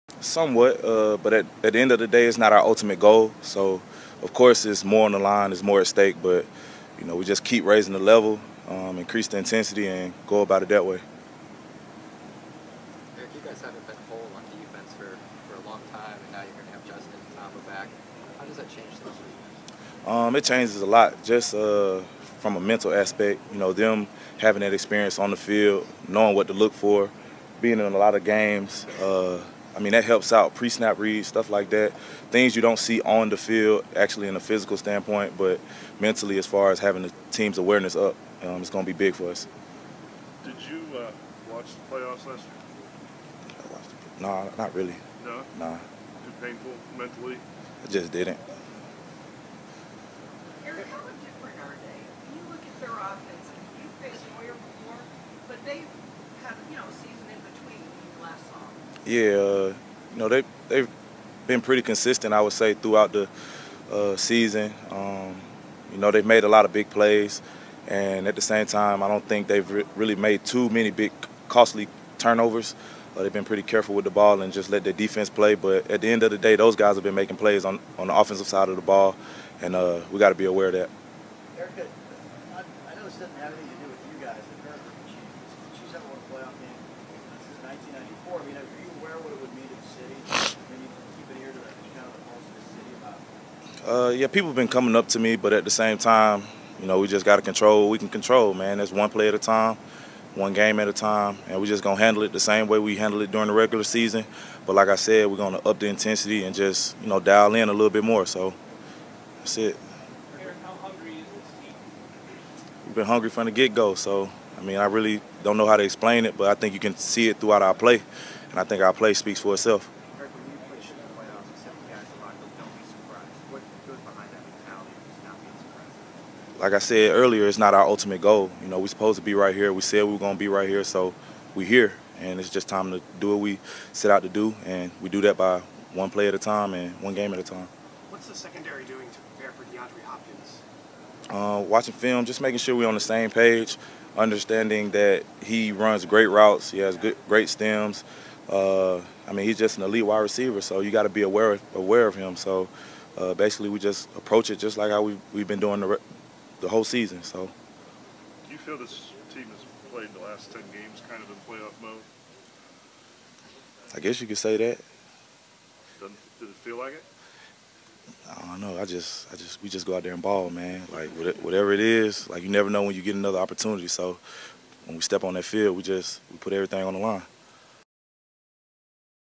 Chiefs S Eric Berry on preparing for the Texans
Kansas City Chiefs safety Eric Berry spoke Wednesday about having the entire defensive unit back as a whole and preparing for the Texans.